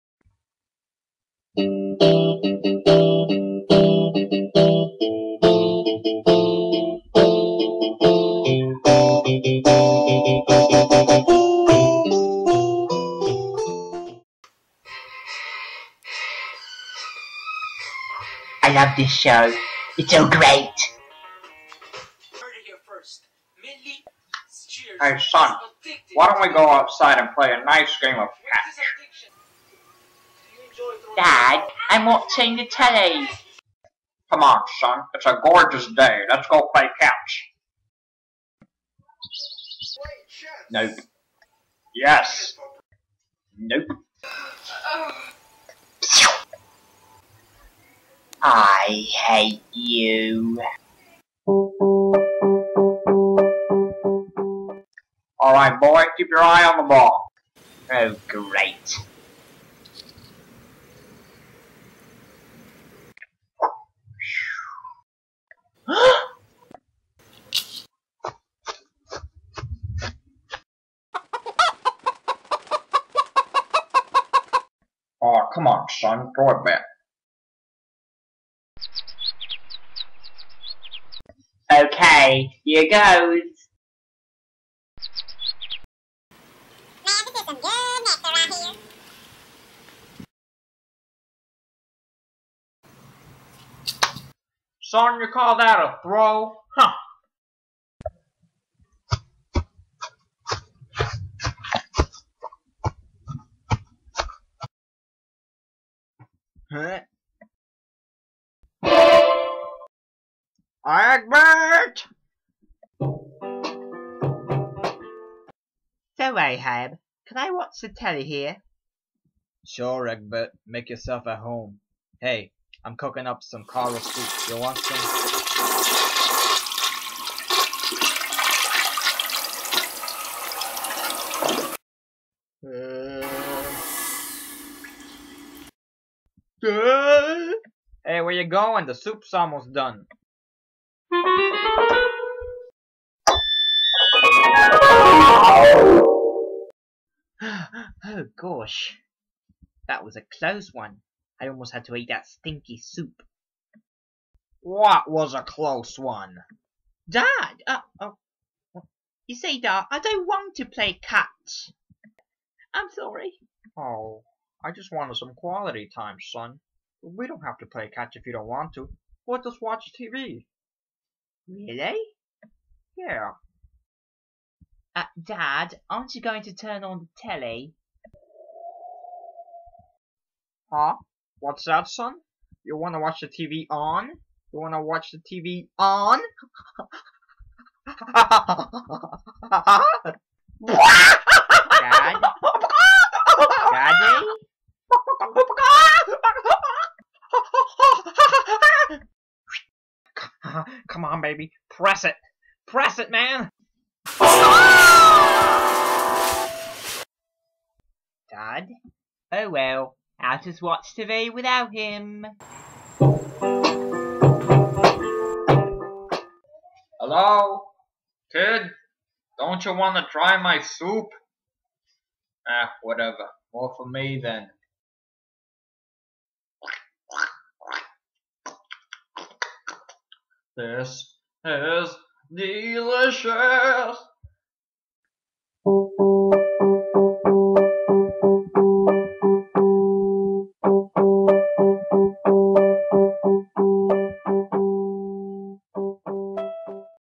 The Wacky Adventures of Eggbert- PILOT
The pilot for my show, The Wacky Adventures of Eggbert. In this episode, we meet Eggbert and his father. His father wants to play sports with him but lazy Eggbert just wants to watch Public Television.